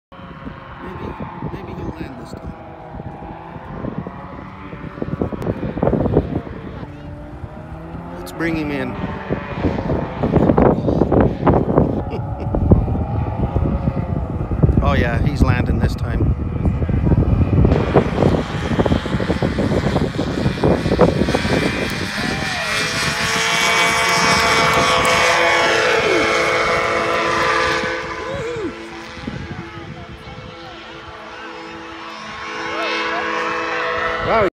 Powered parachute lands and takes right back off again
This is a powered parachute to Cedar